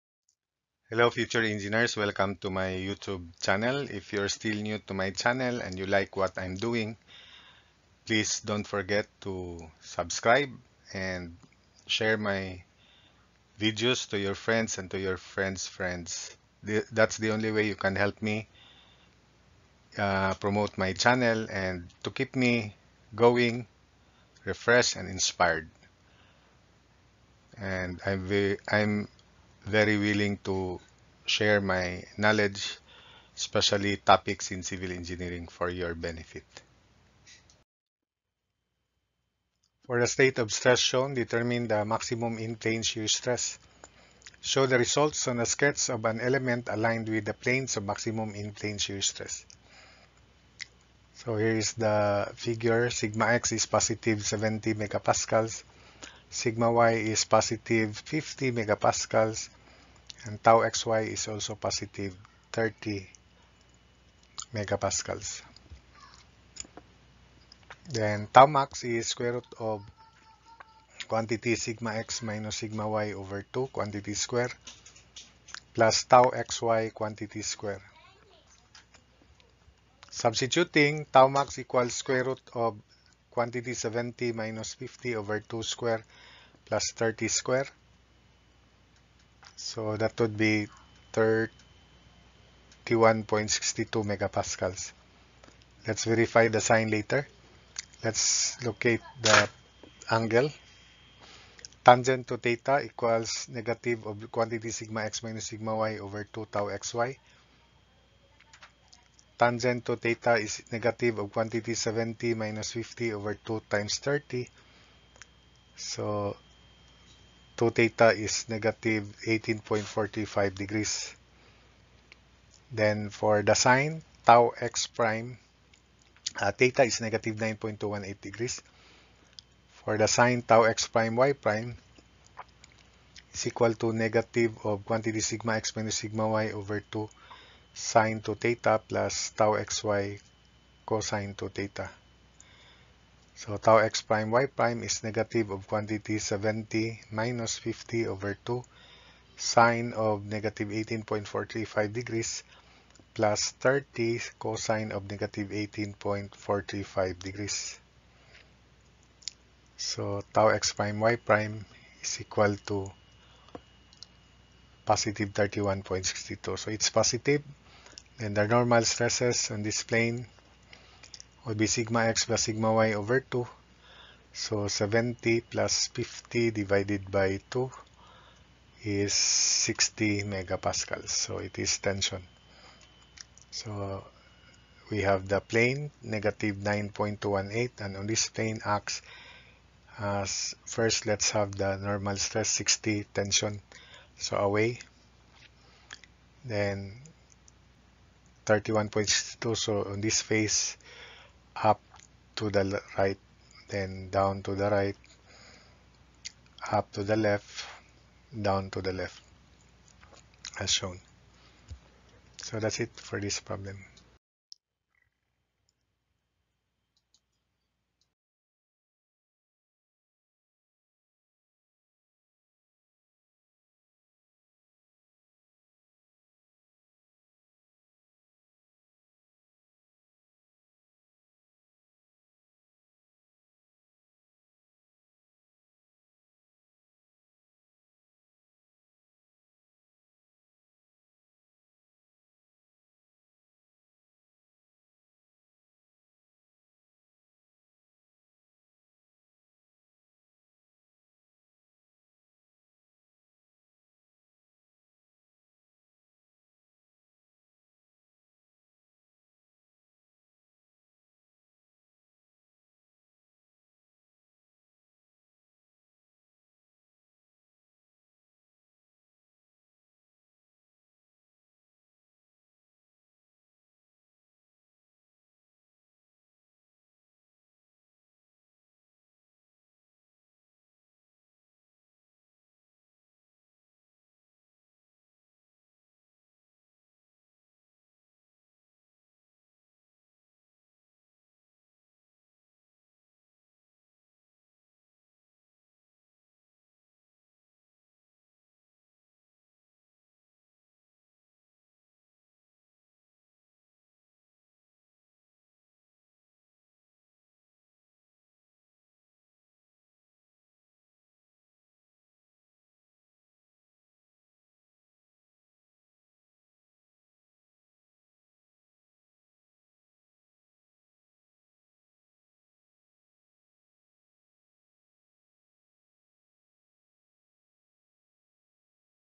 MAXIMUM IN PLANE SHEAR STRESS, Examples 8.44 and 8.45
This video is part of the playlist on the course MECHANICS of MATERIALS that contains lecture and sample problem videos for engineering students.